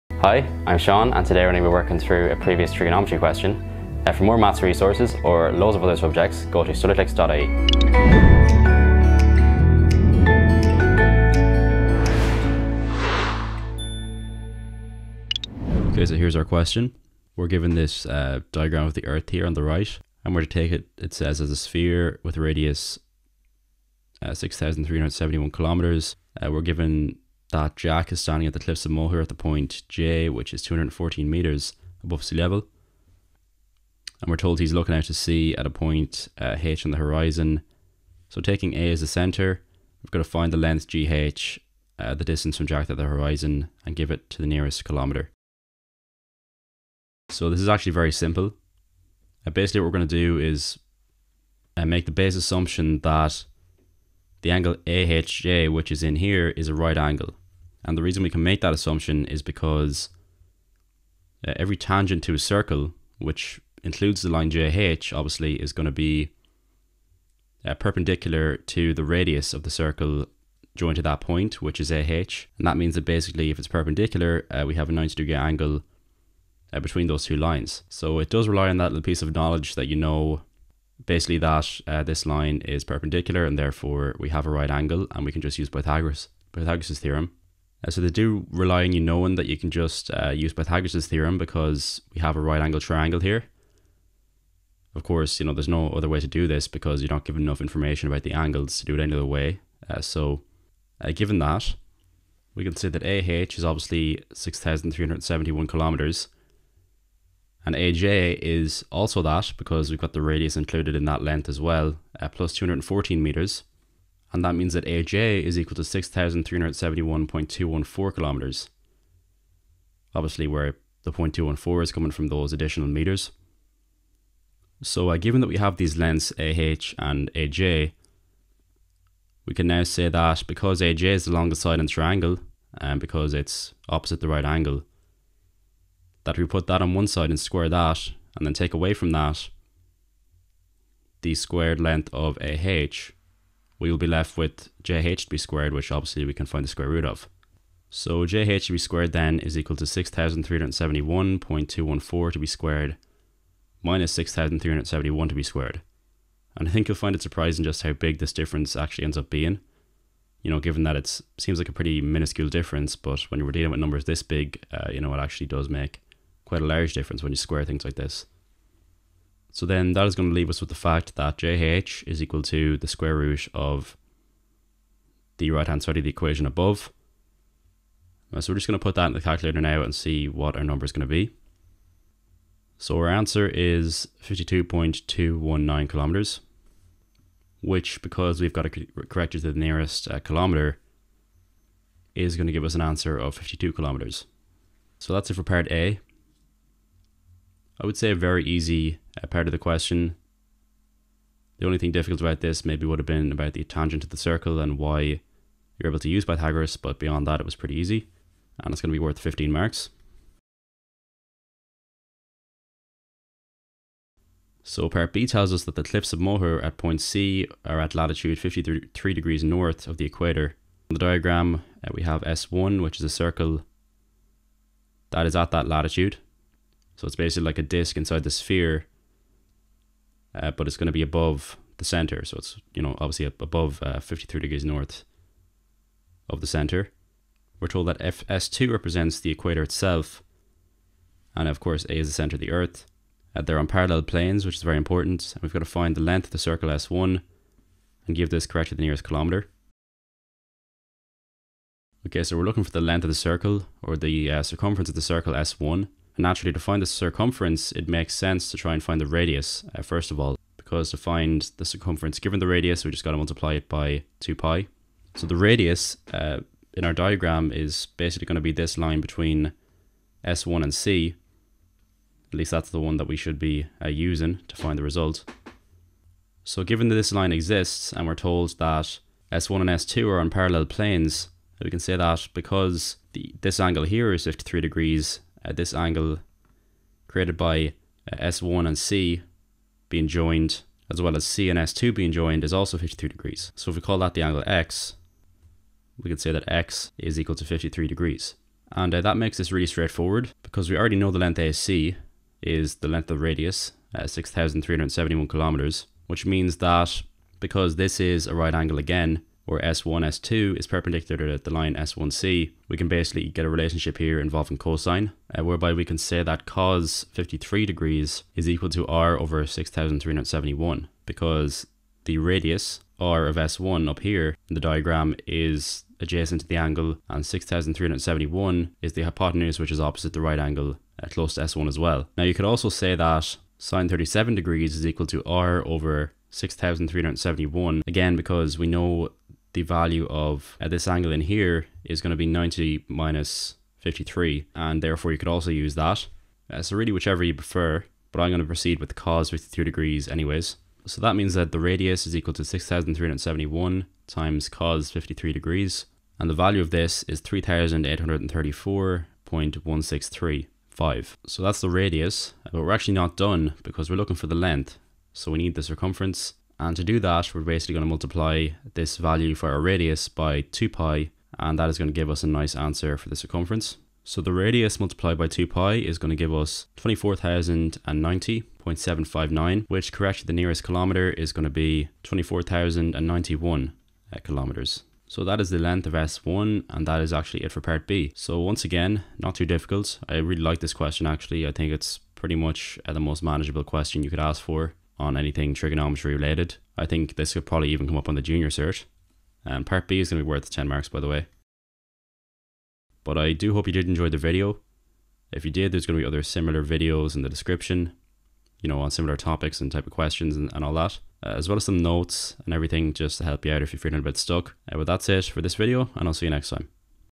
leaving cert maths 2017 paper 2 q6 - Studyclix Maths Solution
Watch Seán walk through this question to show you how to get full marks. For more Leaving Cert HL video solutions, check out